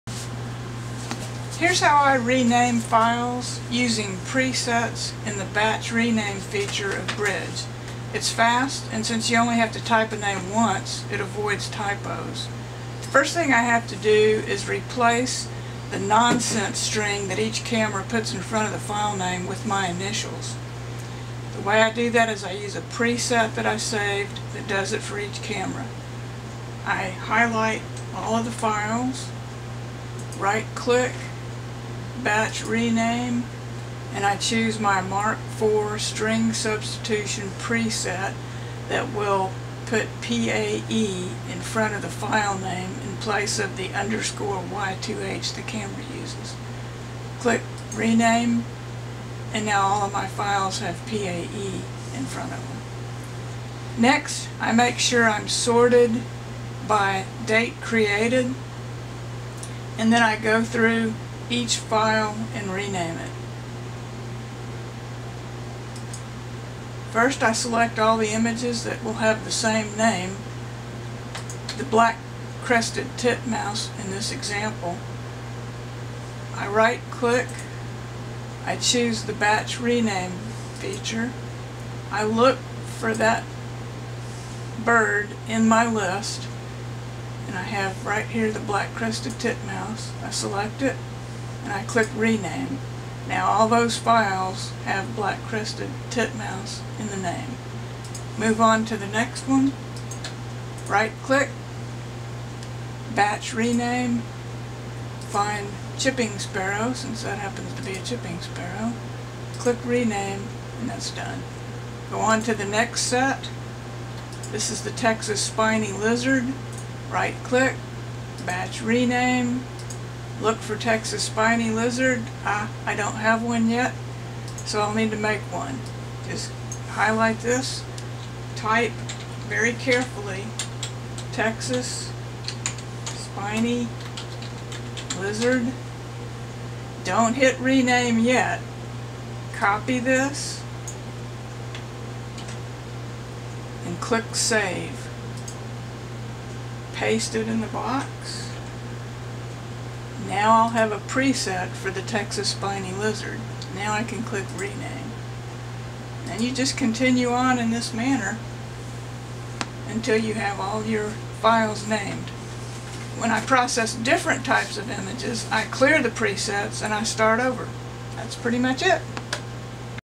Bridge Batch Rename Presets
Naming image files using Presets in the Batch Rename feature of Adobe Bridge